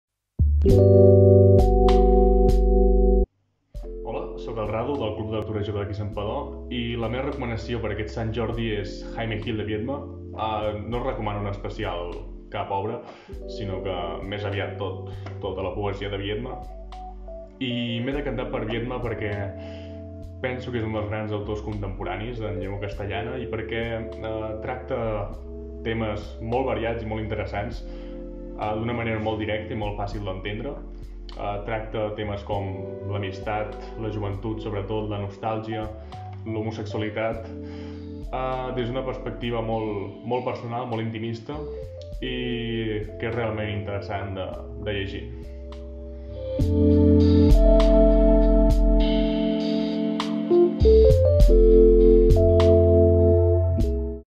Santpedor i el llibre, amb Radu-André Olari
Seguim aquest Sant Jordi tan especial amb noves recomanacions literàries. És el torn de Radu-André Olari del Club de Lectura Jove de Santpedor.
📚Fem d’aquest Sant Jordi tan atípic un espai per compartir idees de llibres per llegir.
👉Feu-nos arribar el vostre suggeriment. Quin llibre us ha agradat i qui n’és l’autor/a

BONA LECTURA!